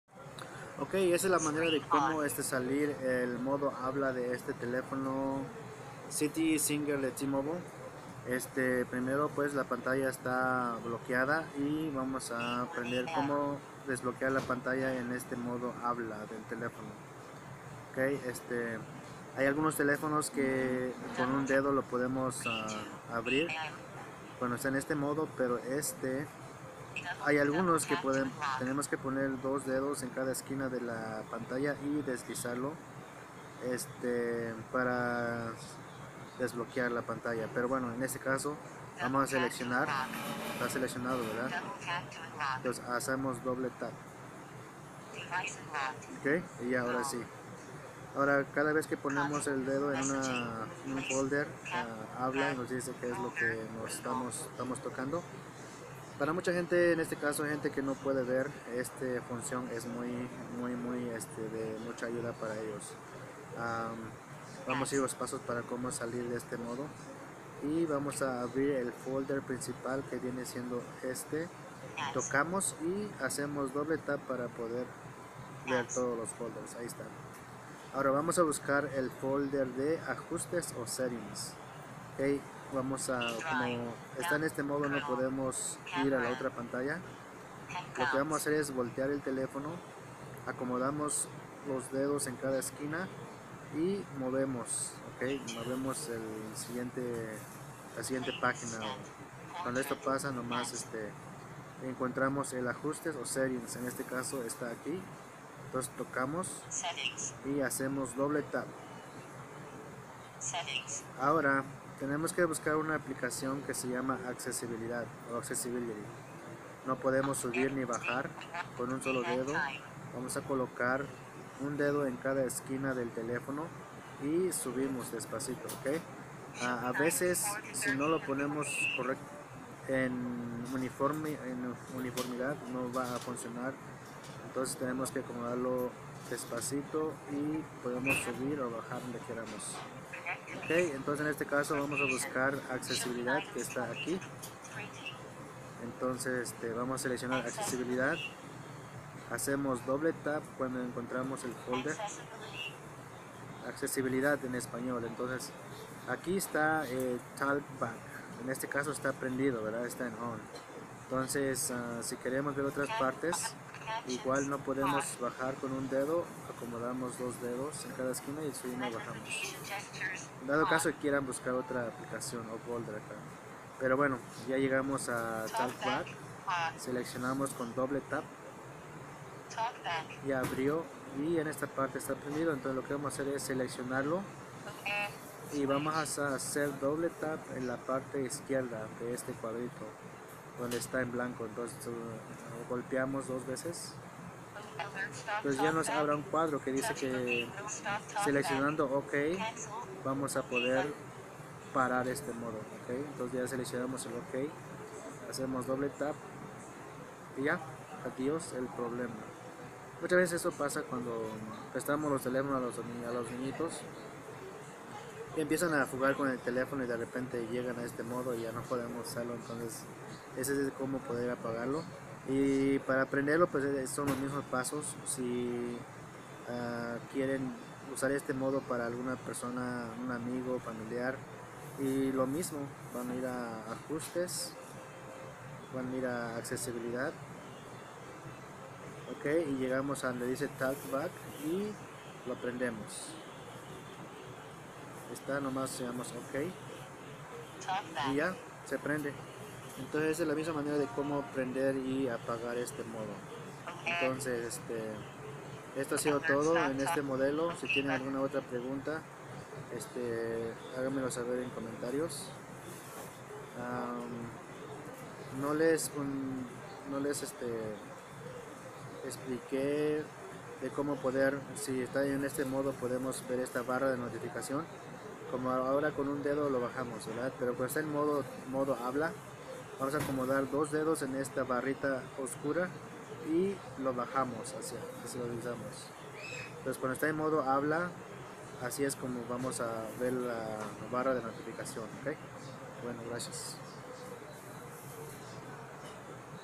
Telefono Habla al tocarlo ZTE Zinger Z667T T-Mobile La solucion!
Si su telefono habla cada vez que toca un folder o la pantalla. Este video muestra como apagar el modo habla talkback en Accesibilidad y ala vez ensena como usarlo o prenderlo para algunas personas que les es muy util.